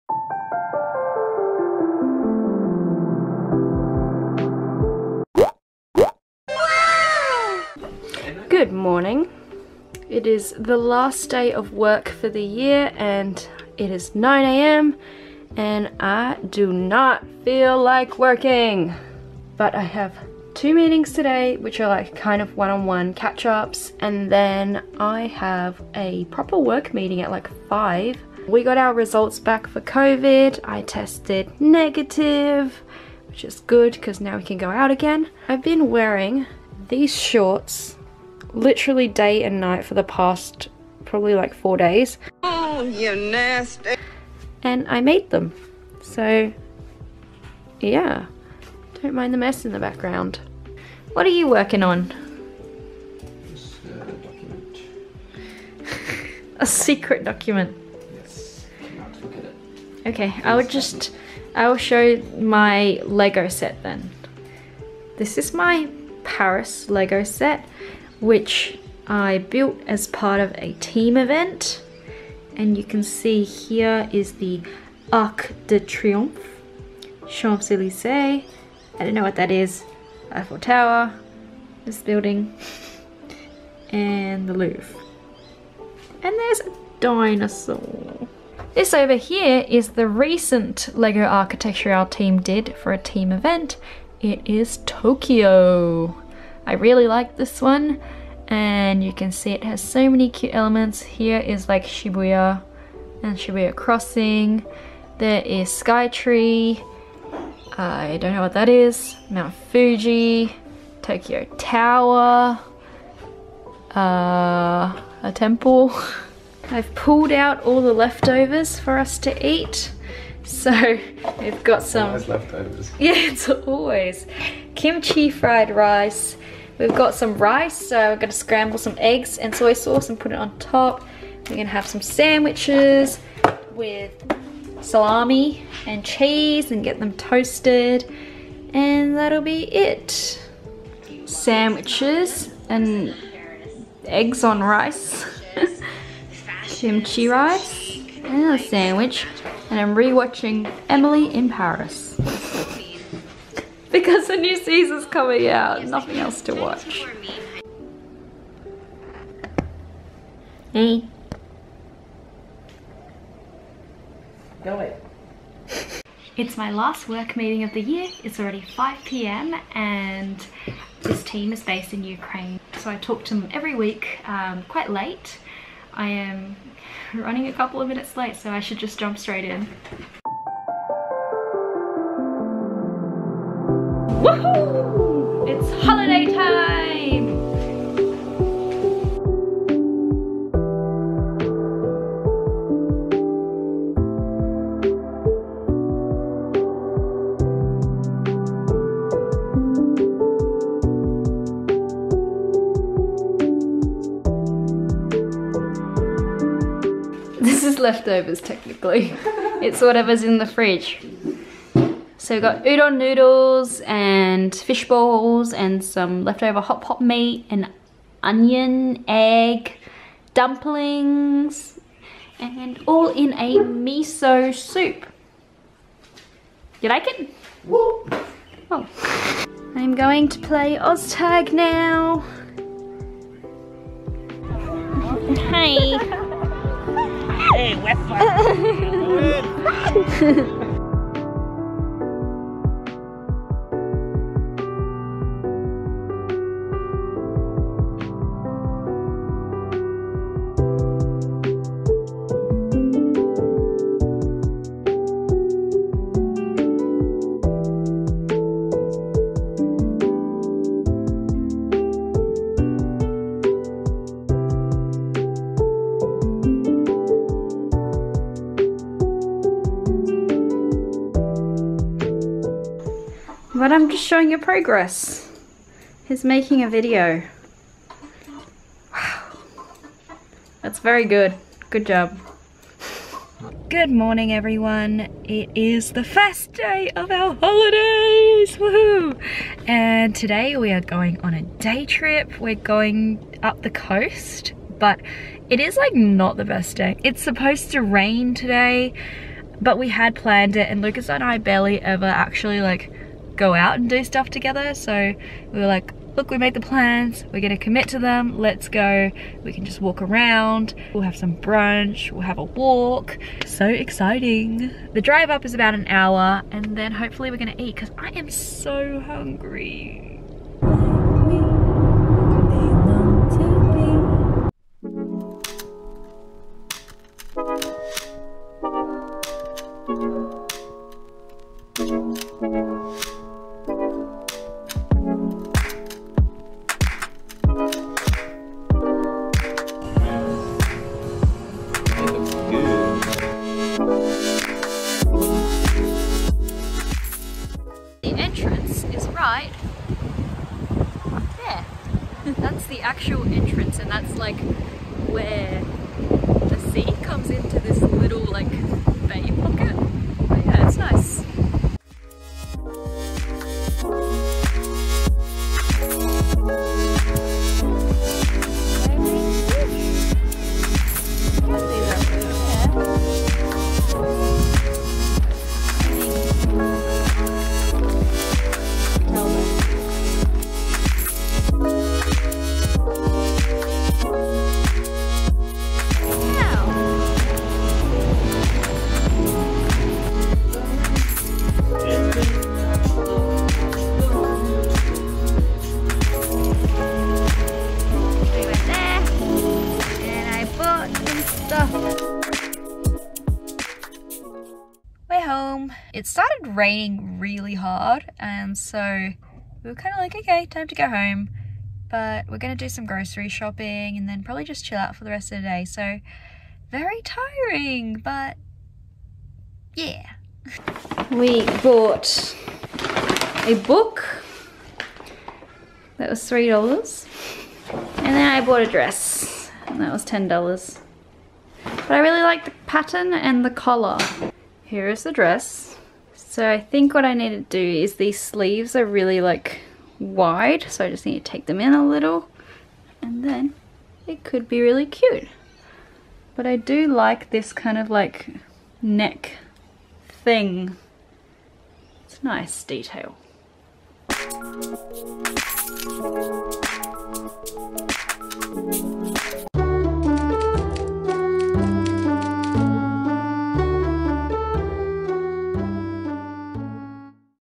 work-life vlogs: last day of work, road trip, thrift finds 🌸 | a day in the life
I've been mentioning in my recent videos that this year I want to continue growing my youtube channel and experimenting with what content you guys prefer. I have been filming a lot of vlogs over the holiday break because i was able to do so much more interesting stuff. In this vlog, I captured the last day of work for 2021 and then Lucas and I went on a day trip to The Entrance, which is this beautiful coastal town about 1hr away from Sydney. We had a yummy brunch, walked along the beach and then escaped downpour by checking out some of the local shops.